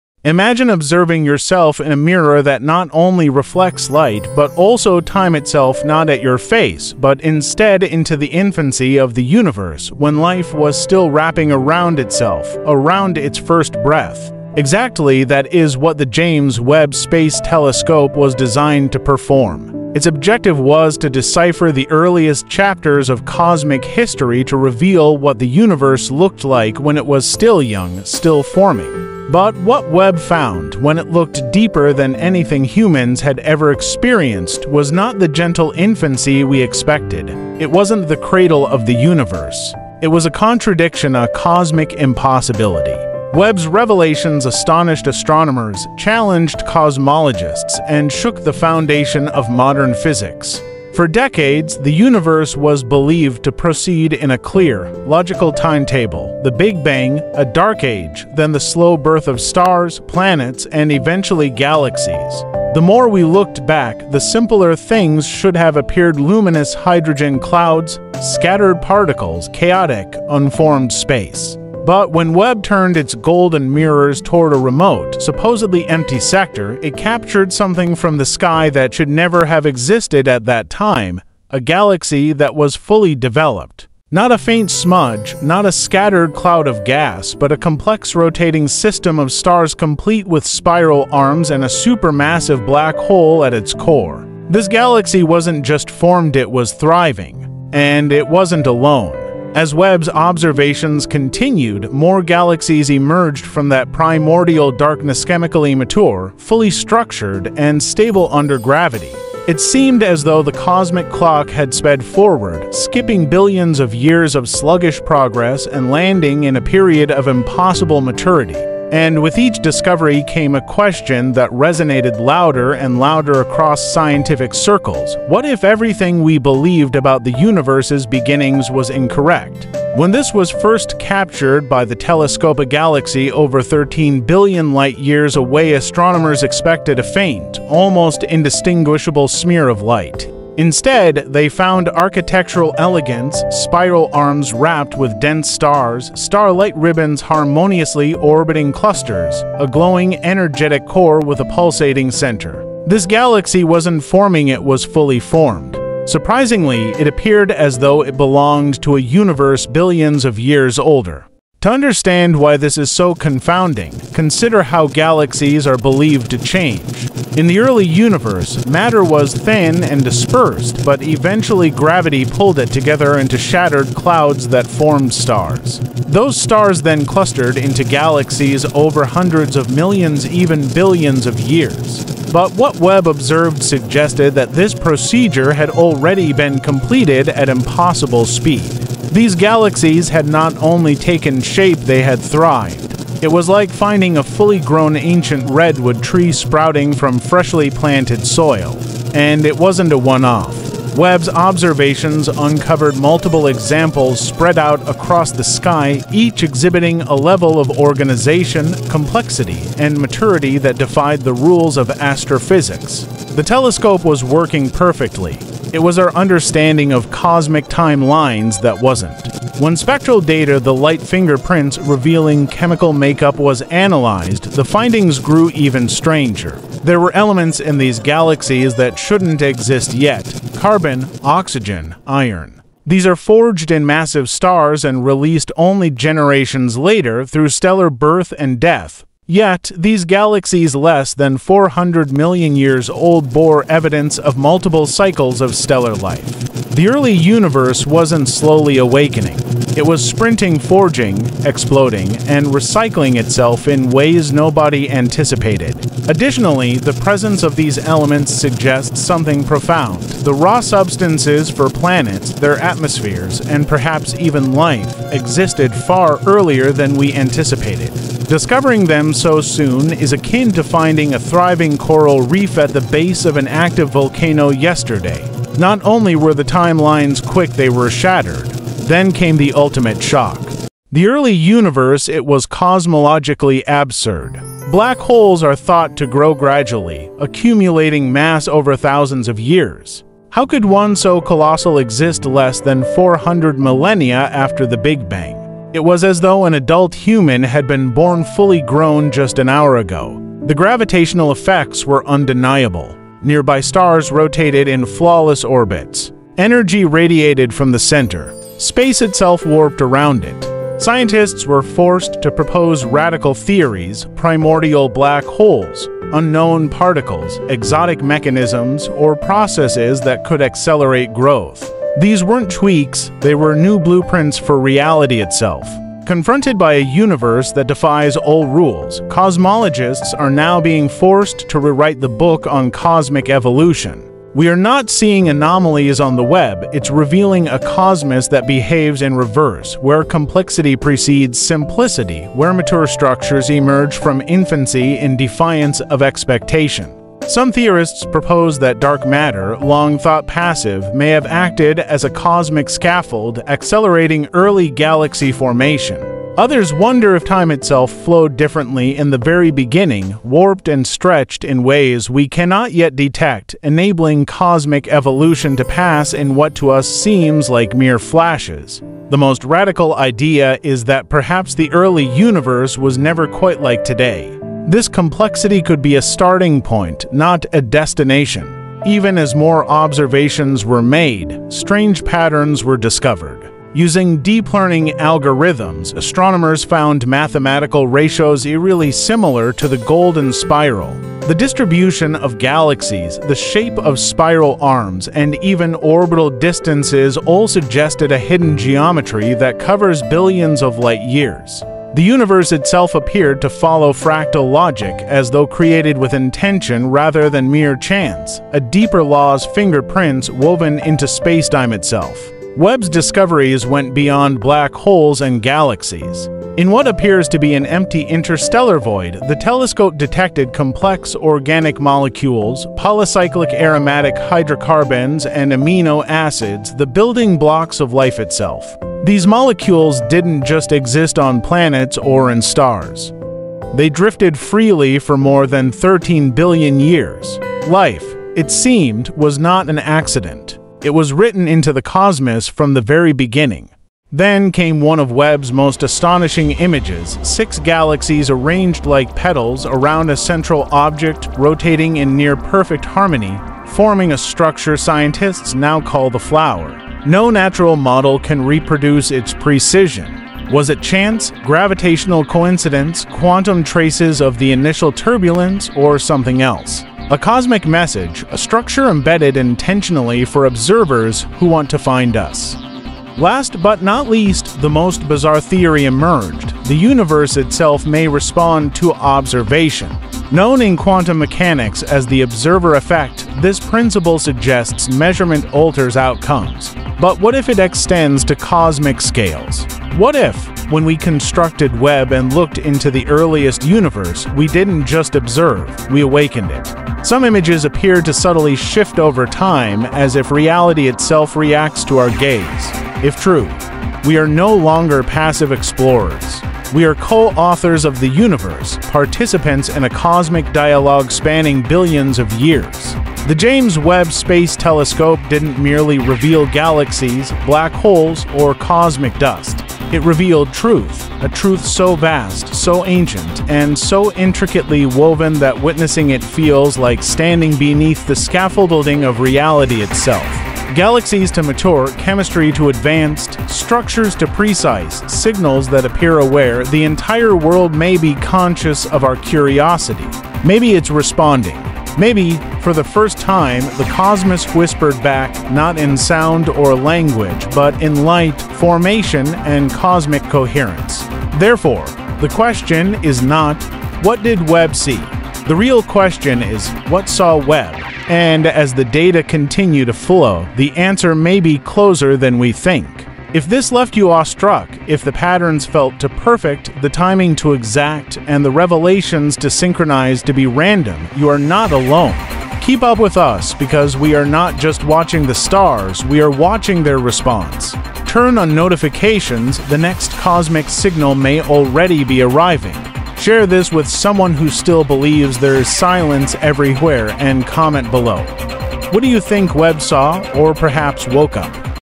James Webb Telescope JUST DETECTED THE UNIMAGINABLE
The James Webb Space Telescope has once again redefined the limits of our imagination. 🚀🔭 Scientists have revealed breathtaking new data that could completely transform our understanding of the universe—shedding light on the earliest galaxies, mysterious cosmic structures, and phenomena we never thought possible. 🌌✨ Humanity is peering deeper into time and space than ever before… and what Webb has found is nothing short of unimaginable.